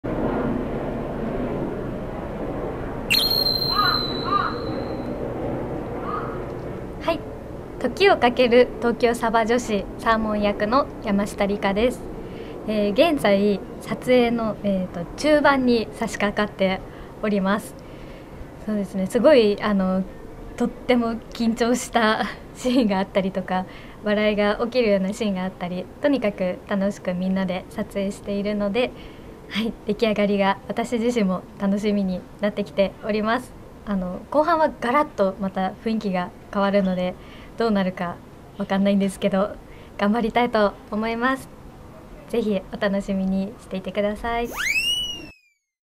【メッセージ】二代目サーモン役・山下りか 【東京サバ女子映画シリーズ】【新作撮影】
東京サバ女子映画シリーズの長編映画「時をかける東京サバ女子」撮影中、主役はこの人です。二代目サーモン役の山下りか。